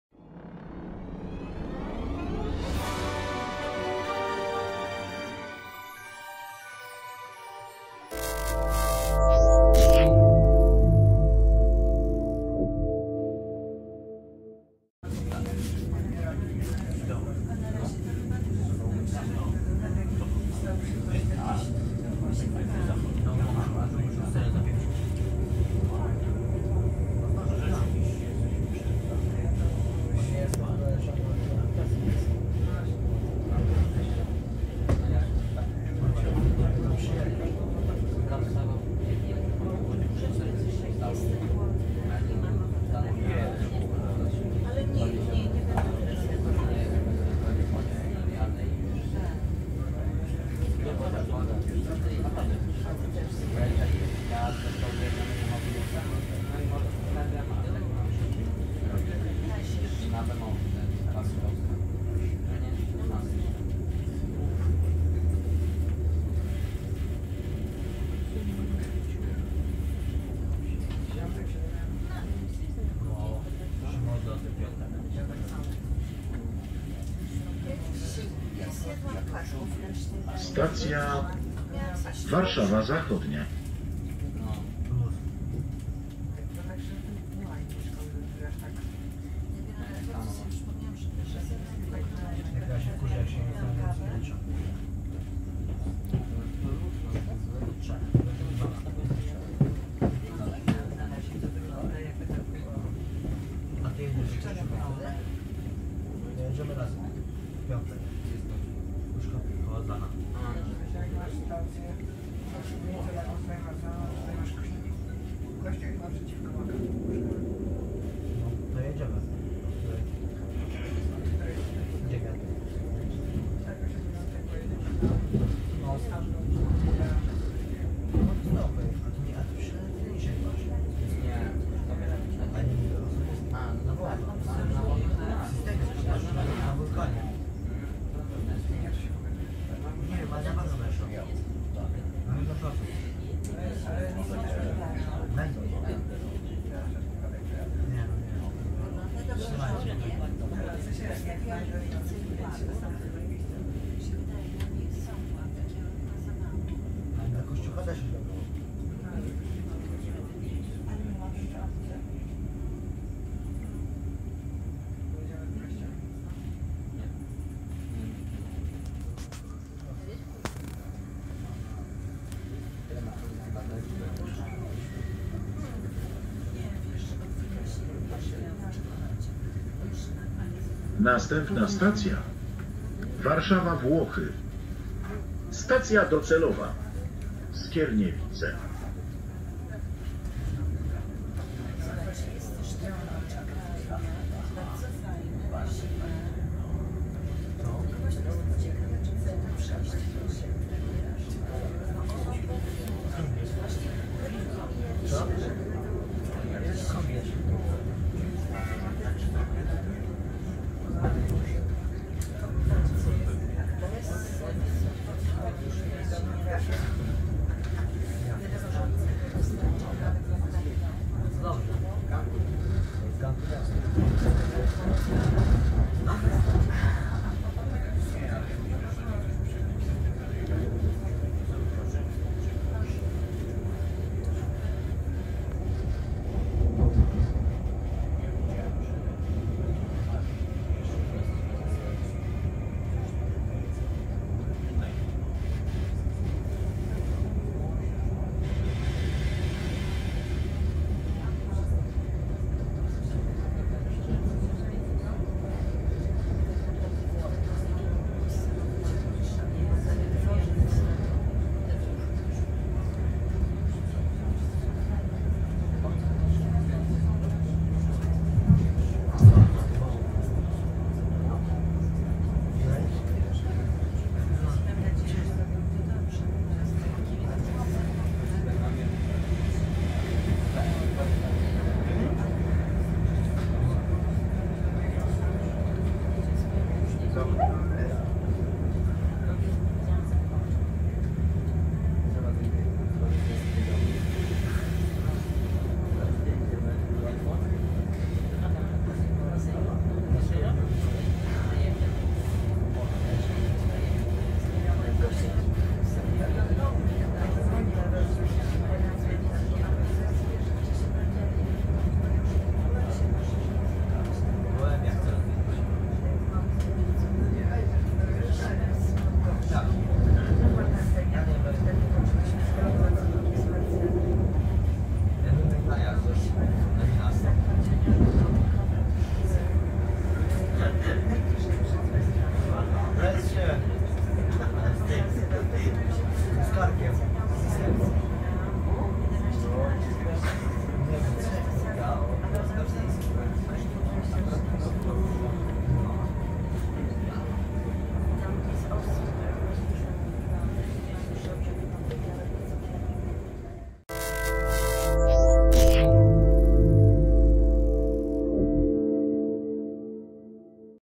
Stacja Warszawa Zachodnia. Nowe tory Przejazd kierunek Warszawa Włochy.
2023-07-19
Nowa Zachodnia ma być zintegrowanym węzłem przesiadkowym łączącym stację kolejową, przystanki tramwajowe, dworzec autobusów dalekobieżnych, zespoły miejskich przystanków autobusowych, postoje Taxi i parkingi dla samochodów osobowych i rowerów. Podziemny przystanek tramwajowy będzie podobny do stacji metra. Perony tramwajowe będą bardzo blisko peronów kolejki WKD i znajdą się na poziomie -2. Na poziomie -1 będzie korytarz łączący wszystkie perony. Pasażerowie nie będą musieli wychodzić na zewnątrz, aby przesiąść się z tramwaju do pociągu.